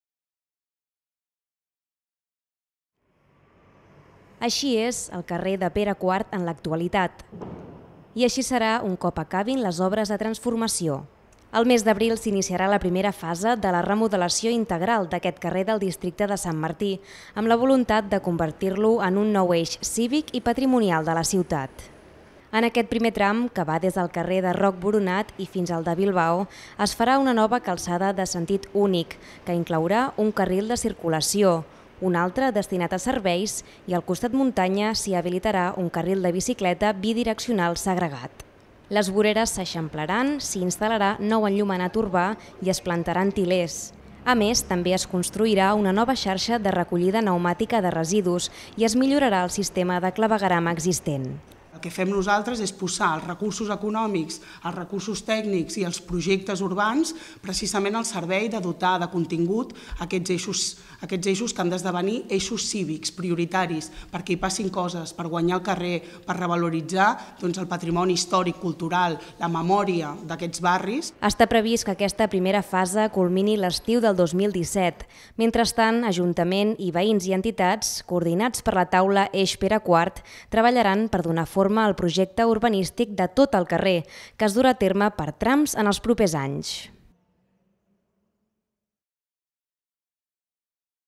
Comença la transformació de Pere IV en un eix cívic
La transformació del carrer de Pere IV, al districte de Sant Martí, ja està en marxa. En una primera fase s'actuarà entre el tram comprès entre els carrers de Roc Boronat i Bilbao. Les obres, que començaran a l’abril i duraran 15 mesos, transformaran el carrer en un entorn més agradable, pacificat i amb prioritat per al vianant. Més informació